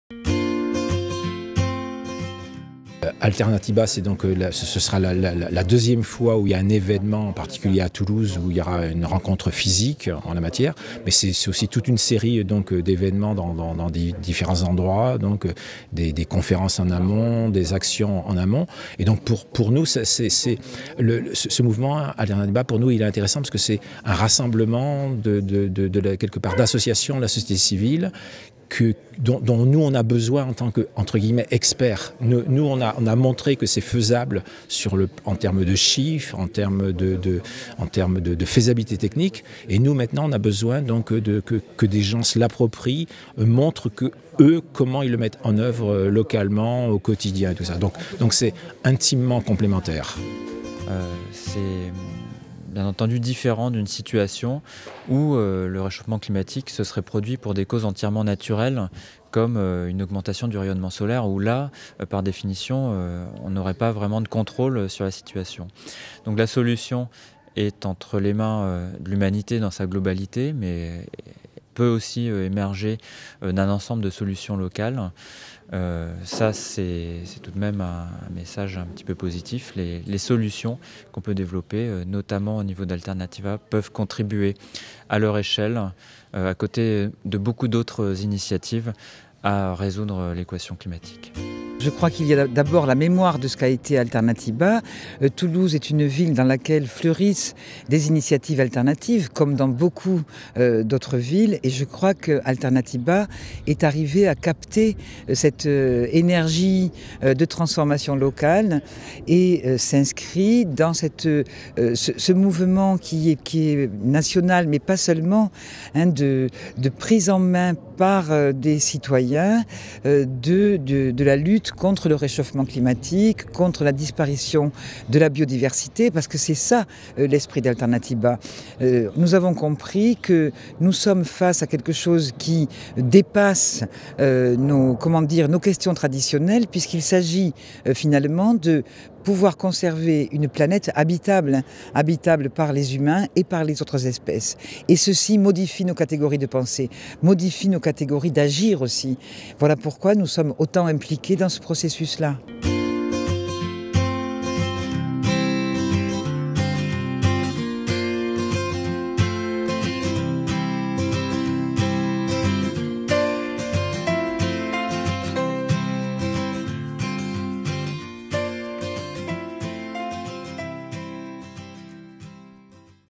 Alternatiba 2017 - Arguments scientifiques
Geneviève Azam, Attac, Paul Neau, Scénario NégaWatt, David Salas, CNRS et Météo France rappellent brièvement les enjeux scientifiques autour du climat et la nécessaire évolution que la société civile doit amorcer. Changeons le système !